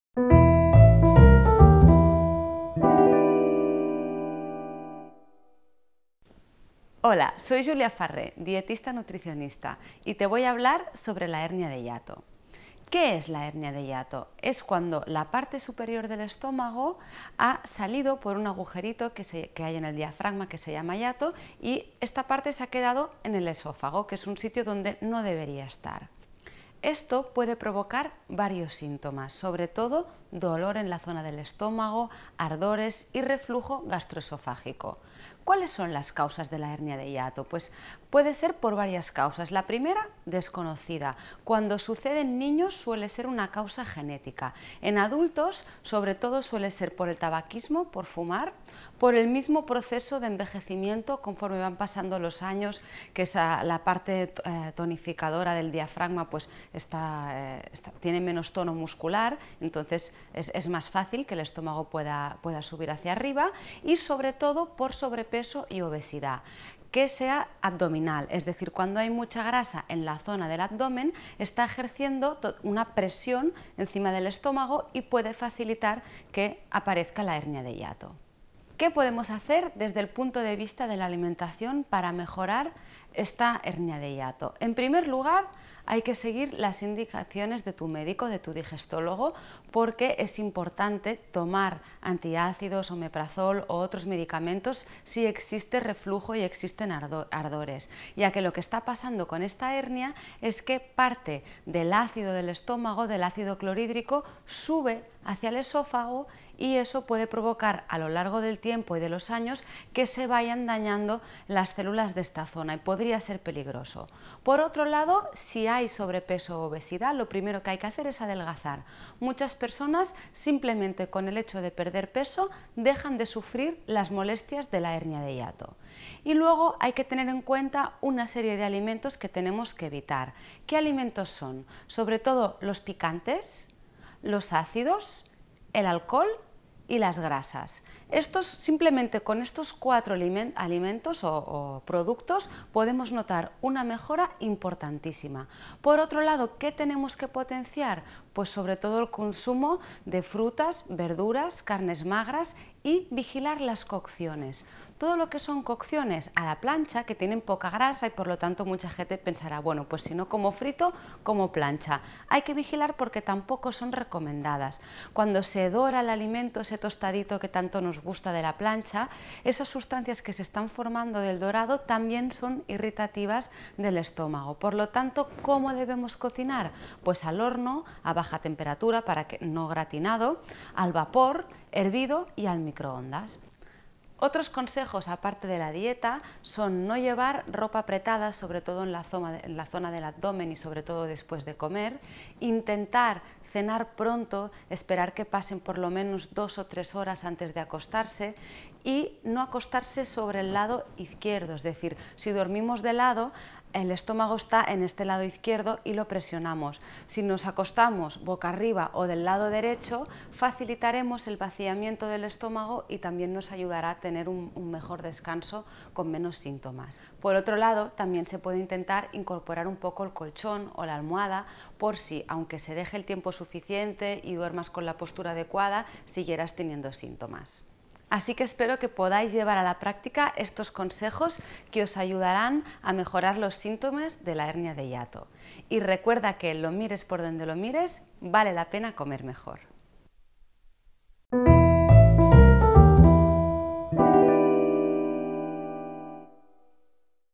🔴 Hernia de hiato y dieta ¿Qué comer para mejorar?
Dieta para la HerniaDeHiato

En este vídeo sobre la hernia de hiato o HerniaHiatal te doy algunos consejos para mejorar la simptomatología que produce. Aproximadamente un 20% de la gente sufre hernia de hiato y muchos no lo saben. Conoce qué es una hernia hiatal, qué molestias produce y cual es la dieta más adecuada para mejorar. Adelgazar es uno de los puntos más importantes, aunque no solamente hacer dieta para perder peso es suficiente para dejar de tener ardores. Hay alimentos que debes intentar evitar por completo y otros que debes potenciar.

Para información más completa sobre la hernia de hiato consulta mi blog